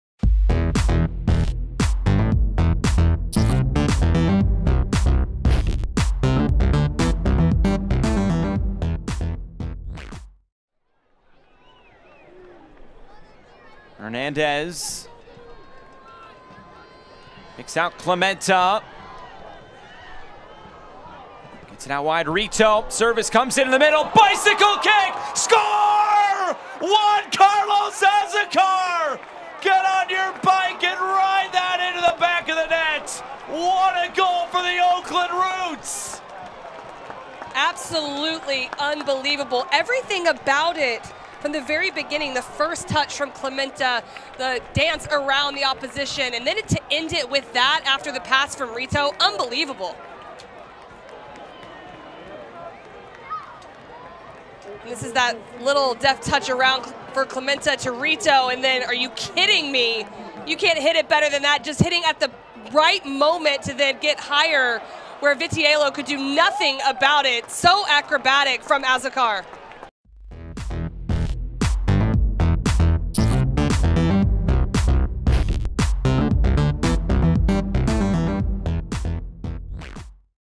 GOLAZO!!! Chilena del venezolano Juan Carlos Azócar
El futbolista venezolano Juan Carlos Azócar se hace notar en Estados Unidos cuando convirtió una perla de gol este domingo. Oriundo de Maracay y con 26 años de edad, ve acción en la segunda división del fútbol estadounidense con el equipo Oakland Roots SC.